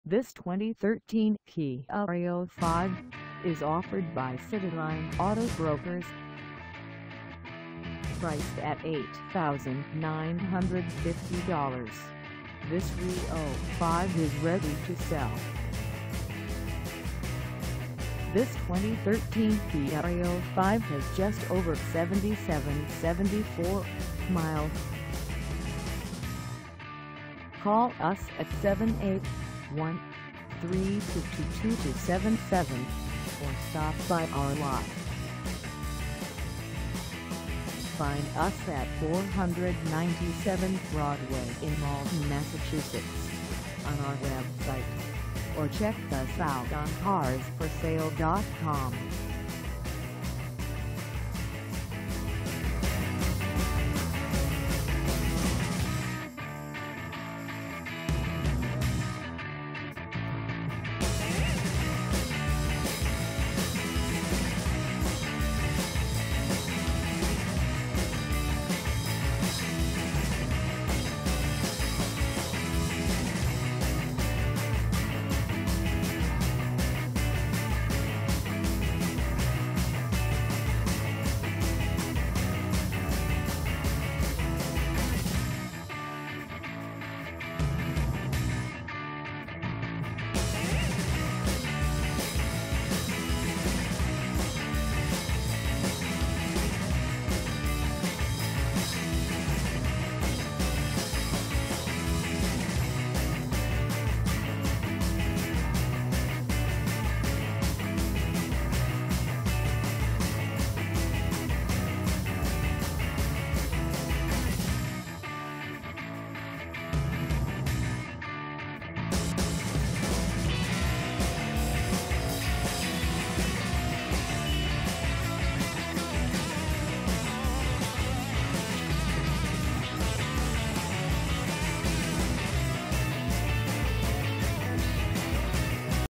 2013 Kia Rio5 LX 4dr Wagon 6A for sale in Malden, MA 02148 a
This 2013 Kia Rio5 LX 4dr Wagon 6A is for sale in Malden, MA 02148 at Cityline Auto Brokers.

Engine: 1.6L I4
Transmission: Automatic 6-Speed
Color: Unspecified
Mileage: 7774
Contact Cityline Auto Brokers 497 Broadway  Malden, MA 02148 to test drive this 2013 Kia Rio5 LX 4dr Wagon 6A today.